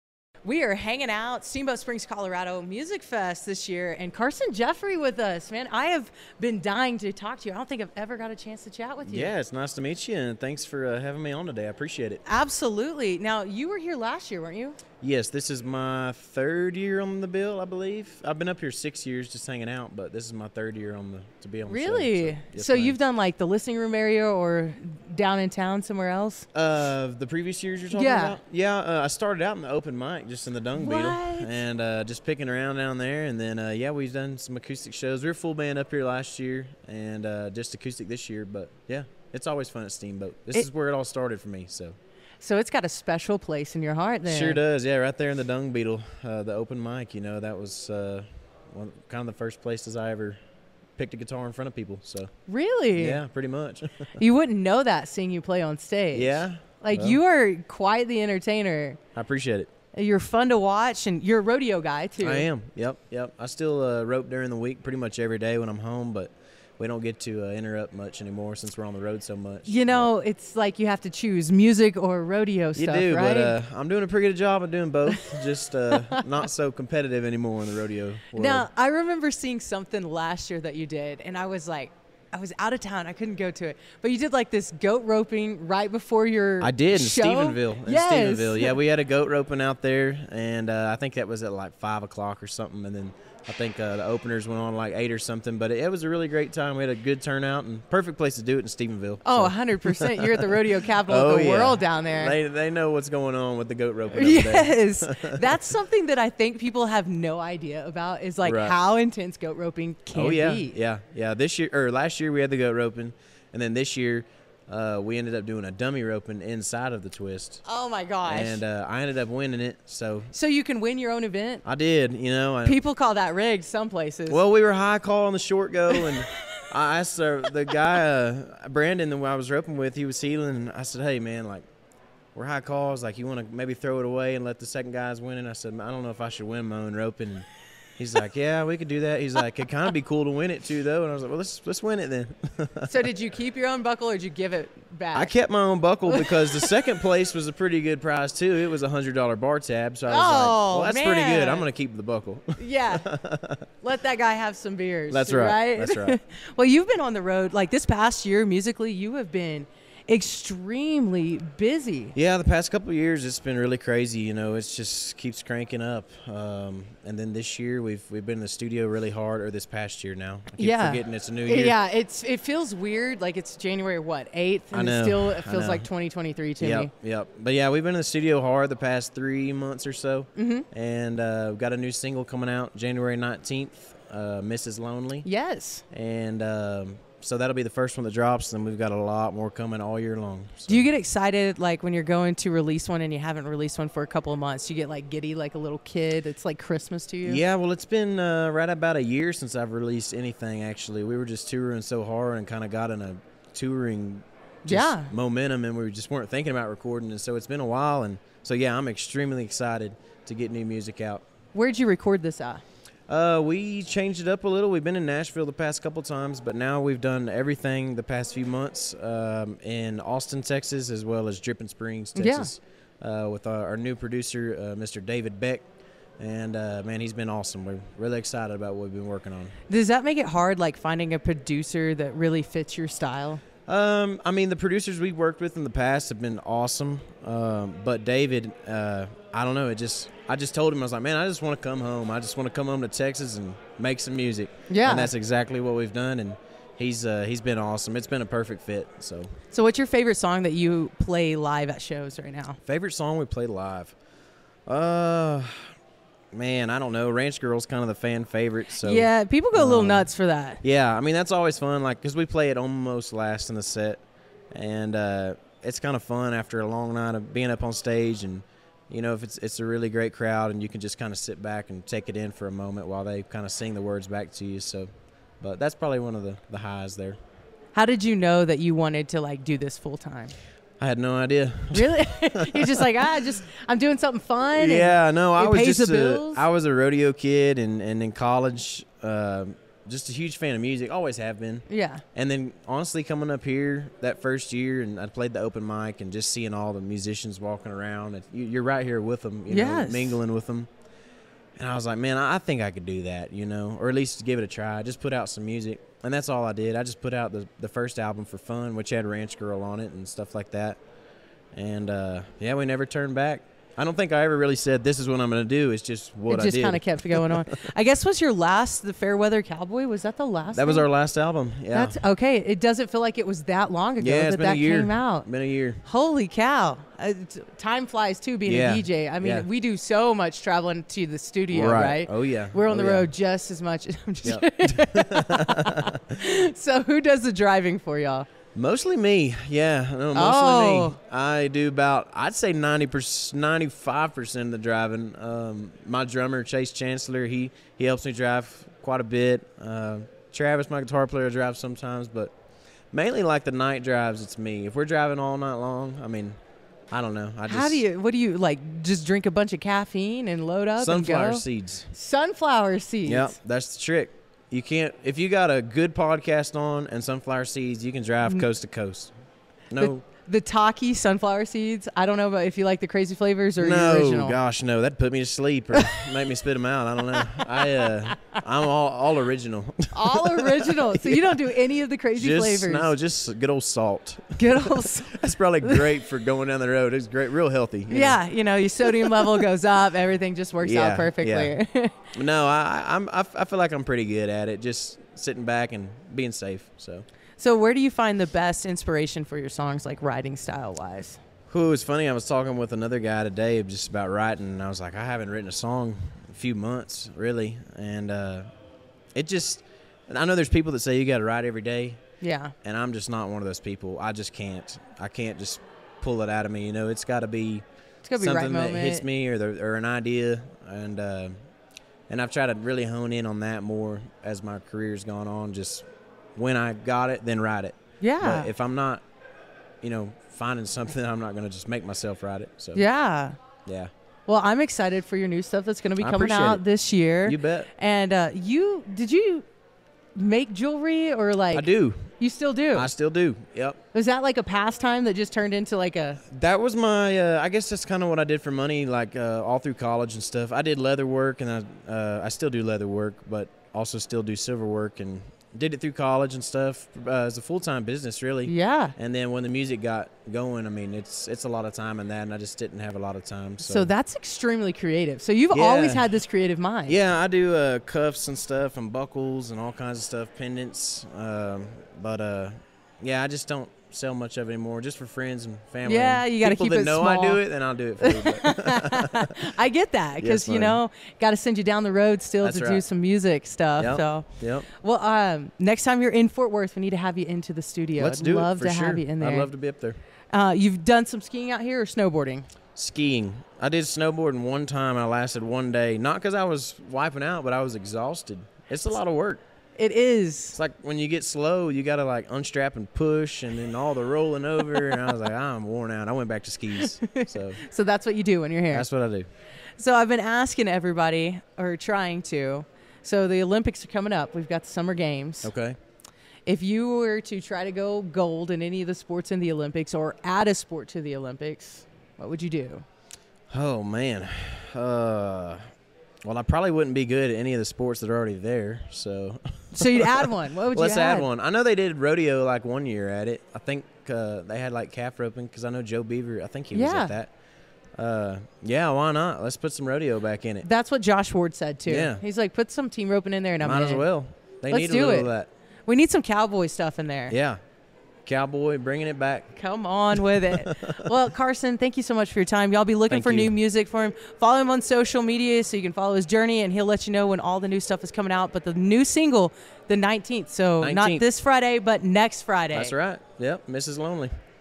Carson Jeffery talks with us at Music Fest 2024
Carson Jefferey had an amazing 2023.  He chats with Heather about what 2024 has in store and NEW MUSIC!!!